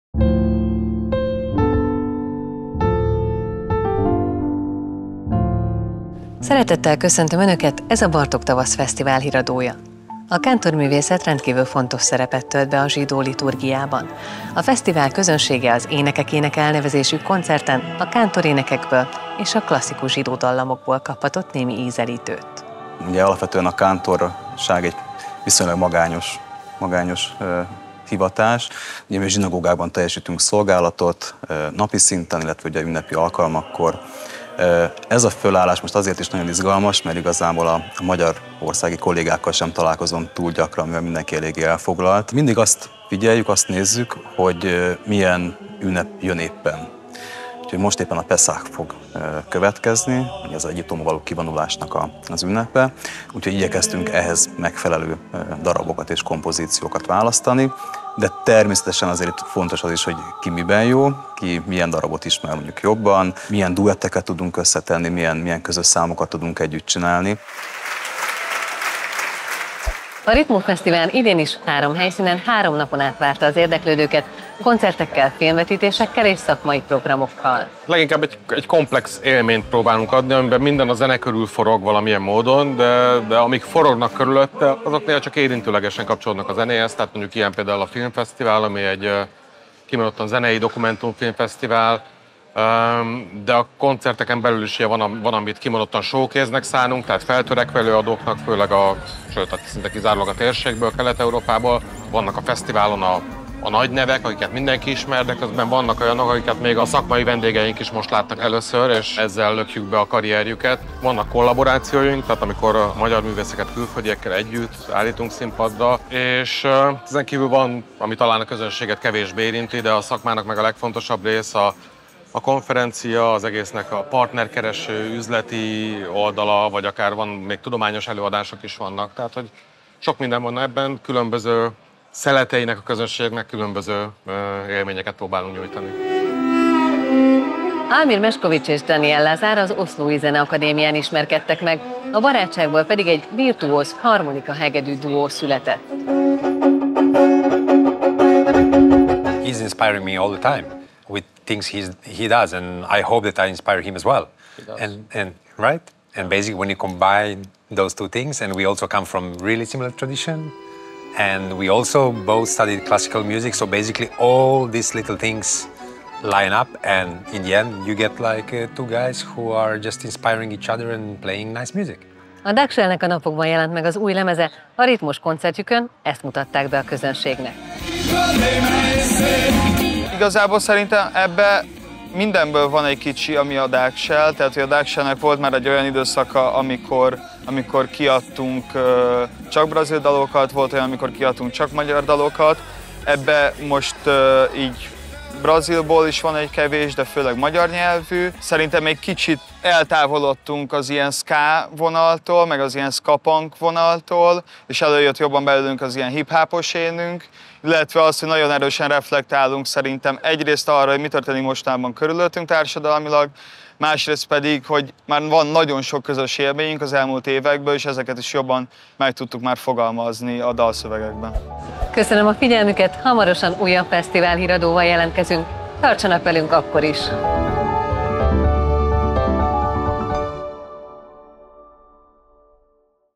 Bartók Tavasz: Fesztiválhíradó – 2024/VII.
Hetedik epizódunkban Seres Gerda és stábunk ezúttal a Müpában találkozott Rudas Dániellel beszélgetett az Énekek éneke c. előadás kapcsán, de ott voltak a Budapest RITMO helyszínein is.

Fesztiválhíradónkban bepillantást nyújtunk a programsorozat pezsgő mindennapjaiba, ellátogatunk a koncertekre, belesünk az előadások kulisszái mögé és beszélgetünk a fellépőkkel, és a közönséggel.
Tartsatok velünk!